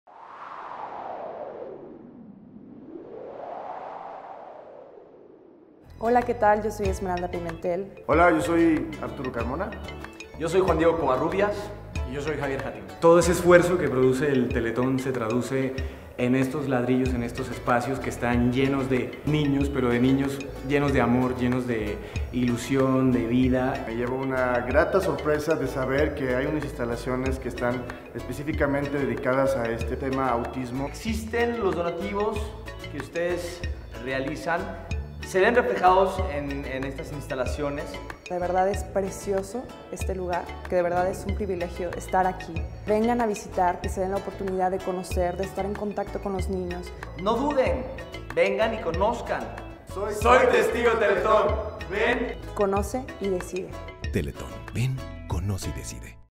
Elenco de la telenovela "La Vecina" - testimonio
El elenco de la telenovela "La Vecina" nos alegró con su visita en un CRIT. ¡Gracias por venir!